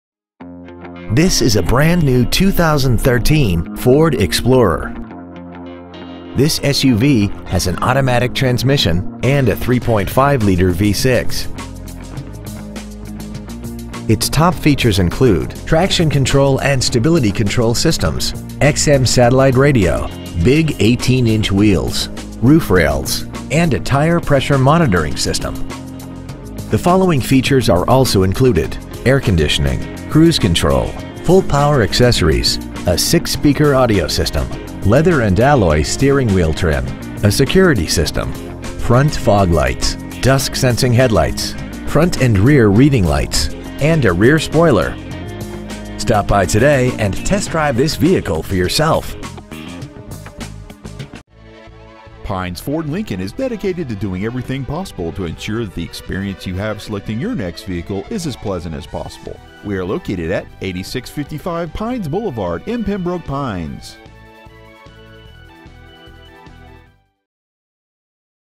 2013 FORD EXPLORER Pembroke Pines FL
Year : 2013
 Make : FORD
 Model : EXPLORER
 Engine : 3.5L V6 24V MPFI DOHC FLEXIBLE FUEL
 Trans . : 6-SPEED AUTOMATIC
 Exterior : WHITE
 Miles : 10
 Interior : LIGHT STONE
 Stock : DGB68385